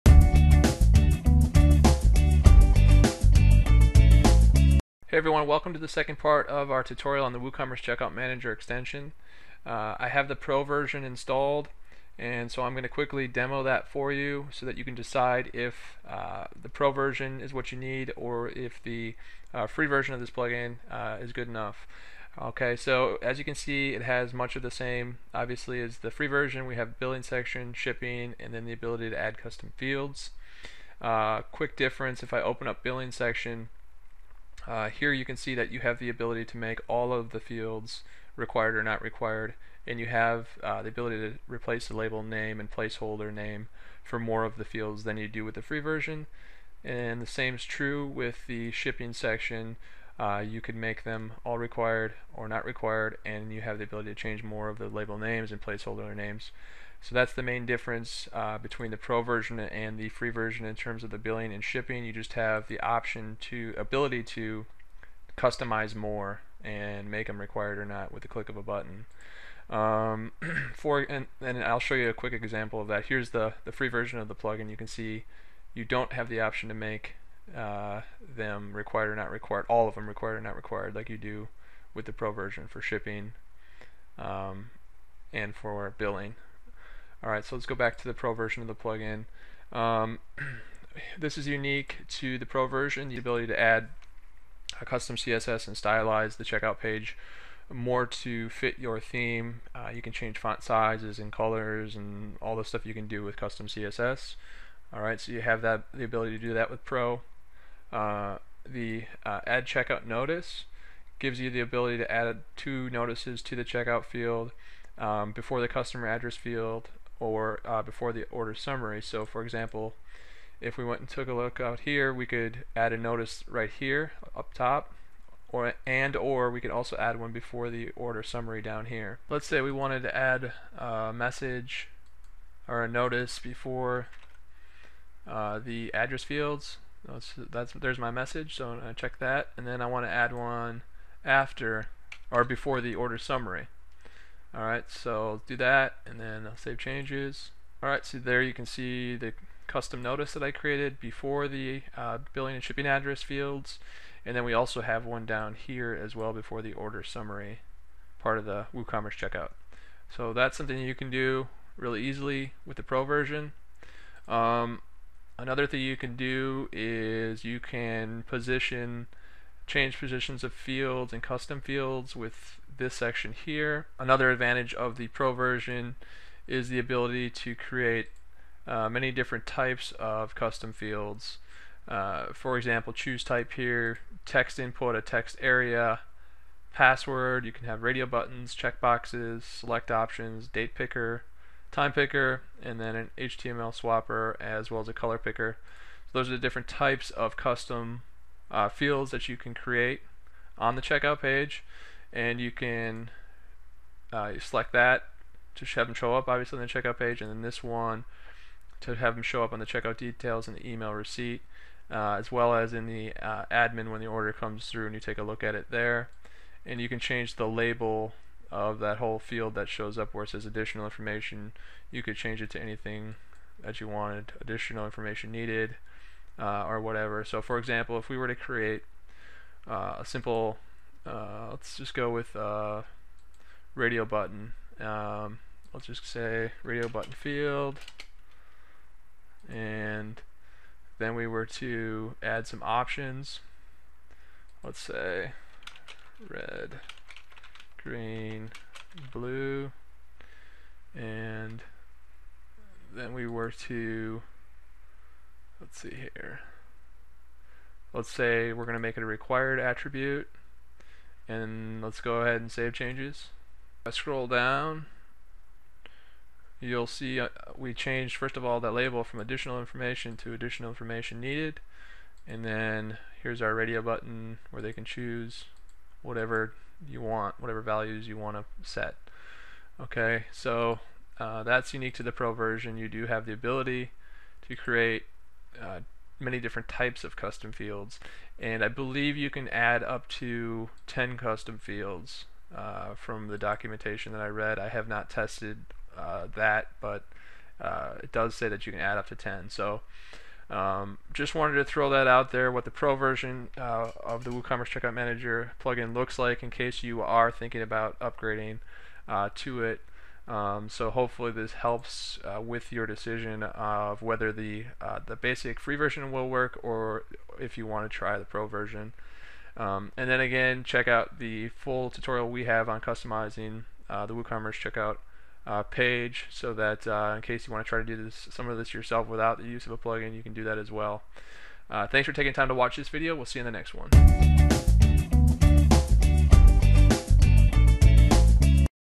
WooCommerce Checkout Manager Pro tutorial - Part 2
In this video we'll demo the Pro version of the WooCommerce Checkout Manager extension.  As you'll see, the premium version of this plugin gives you the ability to customize more aspects of the WooCommerce Checkout page without touching any code.